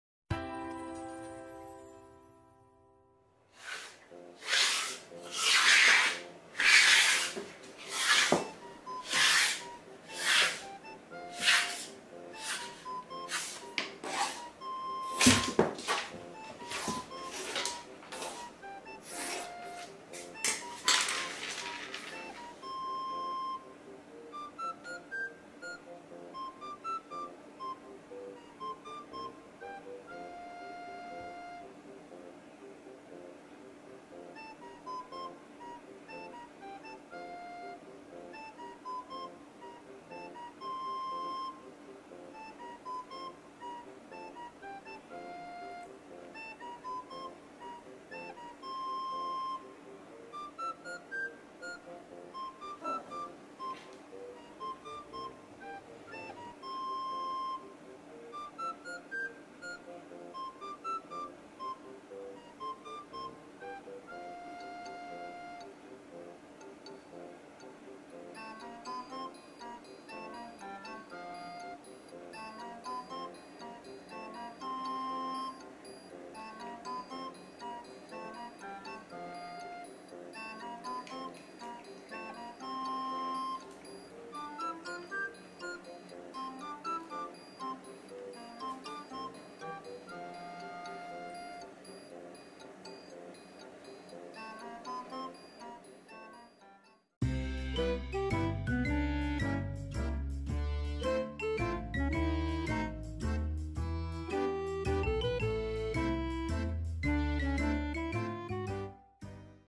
割り箸を見失った後のリアクションが面白い猫。
茶トラ猫の「ちゃい」と、サバトラ猫の「すし」は割り箸遊びが大好きです。割り箸を見失った後のリアクションが面白い猫の動画をご覧下さい。